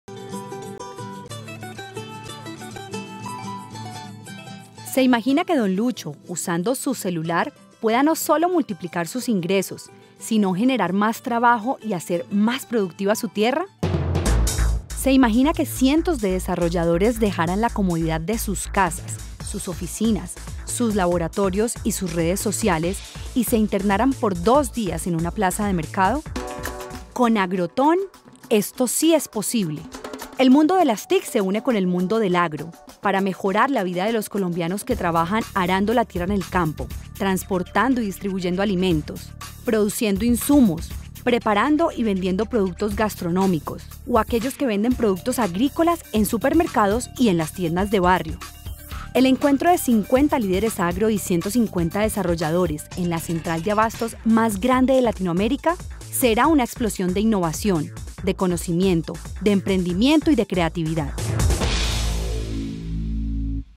En mundo del Agro se encuentra con las TIC
El encuentro de 50 líderes agro y 150 desarrolladores en la central de abastos más grande de Latinoamèrica será una explosión de innovación, de conocimiento, de emprendimiento y creatividad.

Esta experiencia única donde participan desarrolladores, jóvenes rurales, empresas privadas y el Estado, volcará las ideas de miles de colombianos para desarrollar durante 48 horas, 10 aplicaciones para aplicaciones para que don Lucho, María, José, Raquel y miles de colombianos más trabajen en un sector agro más sostenible, eficiente e incluyente gracias al uso de la tecnología.